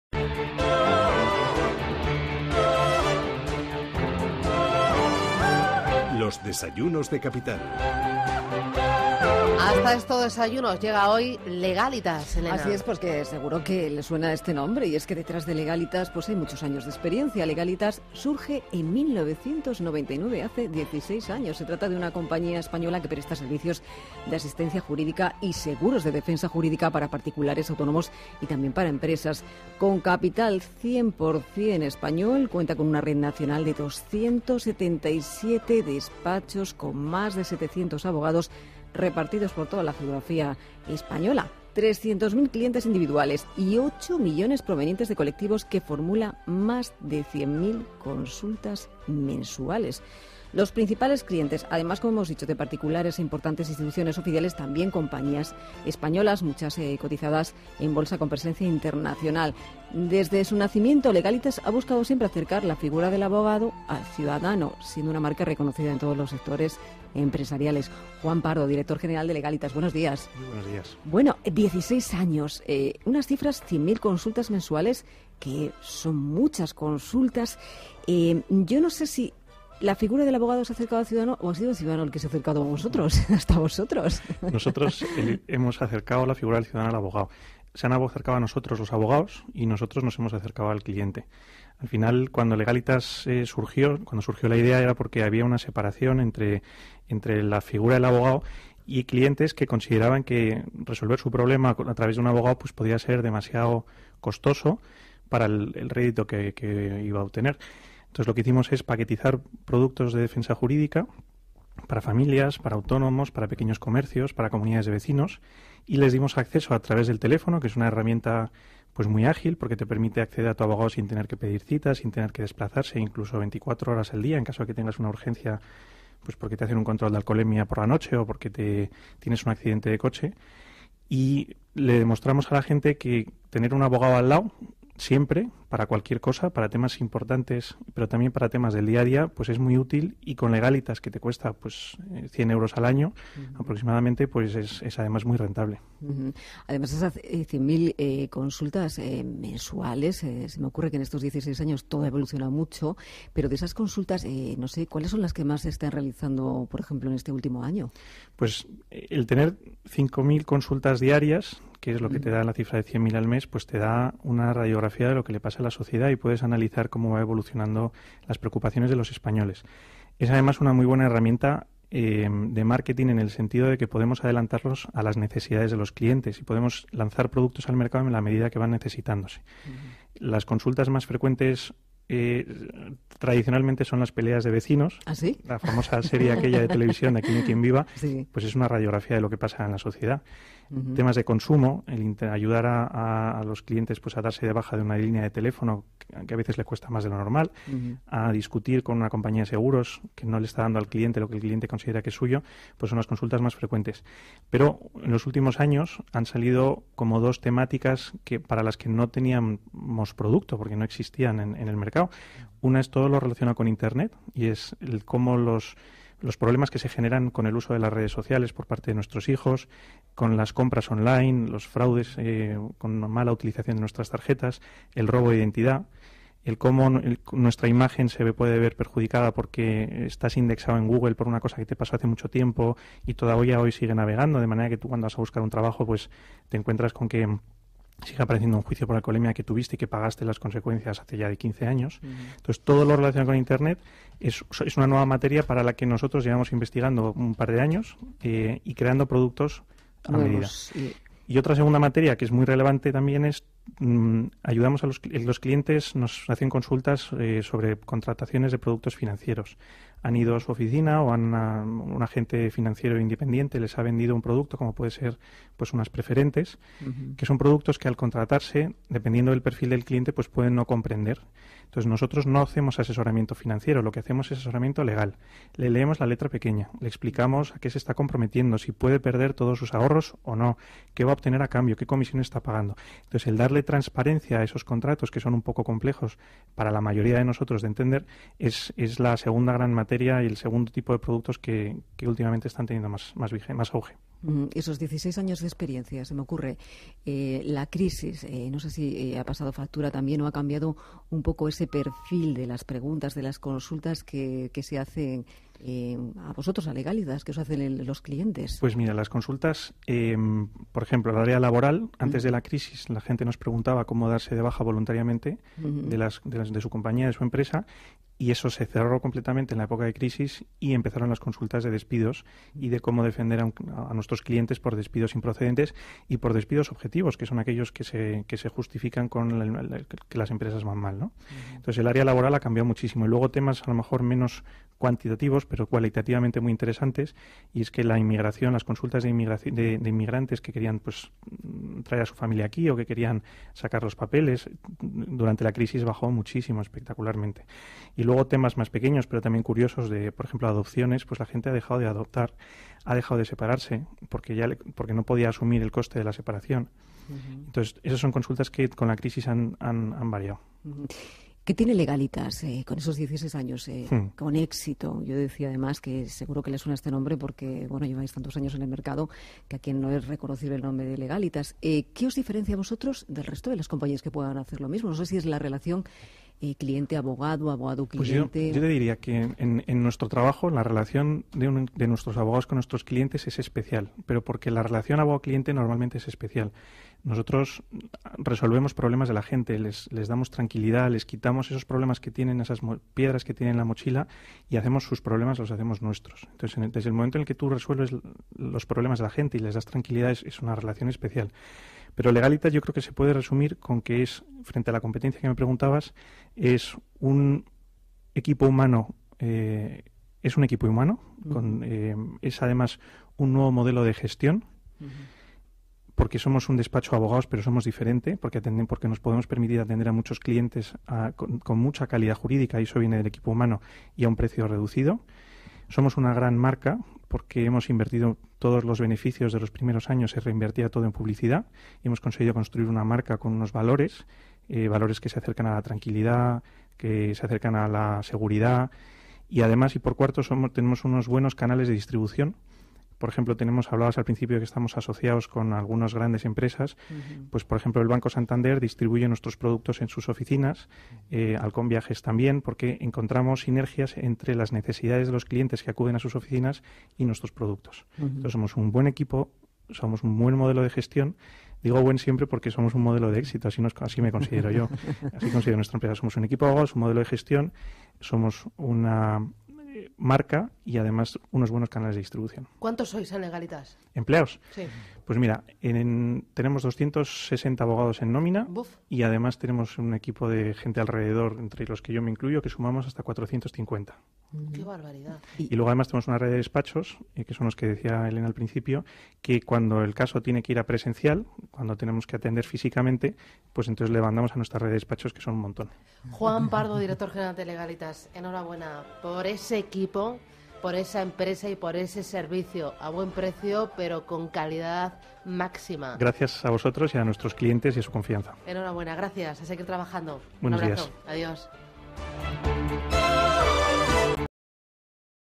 Legálitas en Radio Intereconomía - Entrevista a Juan Pardo
Juan Pardo, Director General de Legálitas, acudió a Radio Intereconomía para hablar de Legálitas y su andadura de estos últimos años.